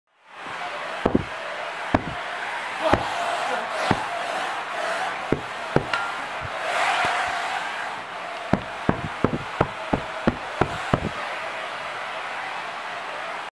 Пыль из сиденья
Смотрите, как удаляем пыль из сиденья